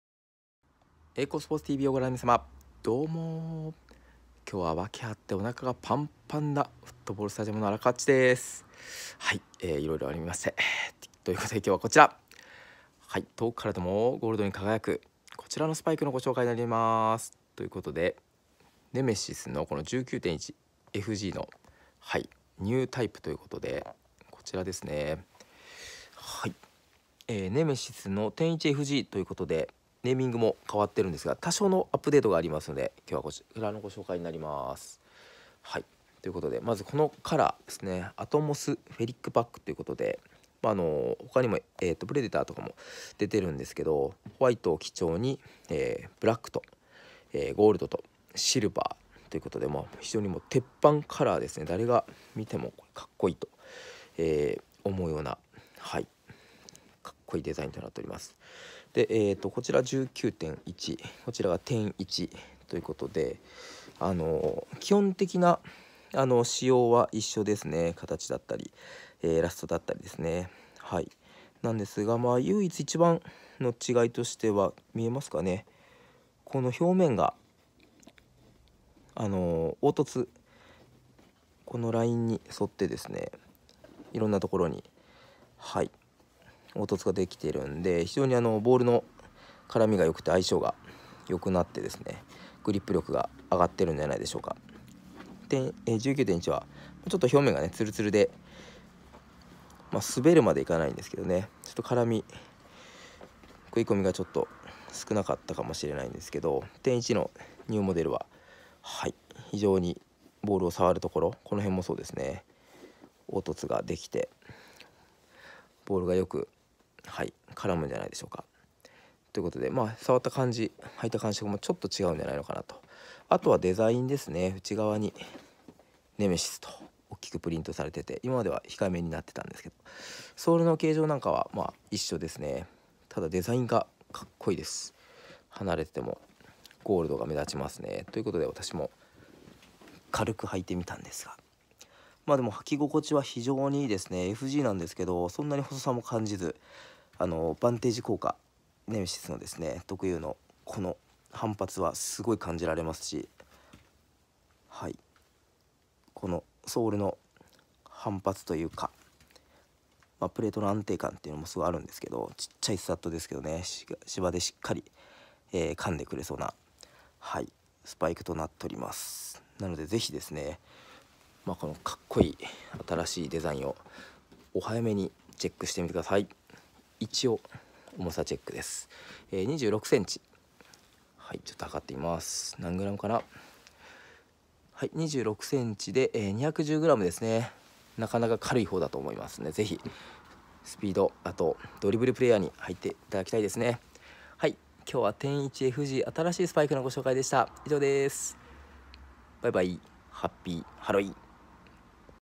岡山 サッカースパイク アディダスサッカー ネメシス
ネメシスのニューモデル、アトモスフェリックパックのご紹介になります！
以前の19.1FGから.1FGへなった事でアッパーに凹凸が出来た事でボールのグリップ性も上がりプレーの質が上がるサポートになると思います。
是非メッシ選手のようなドリブラーに履いていただきたいですね？！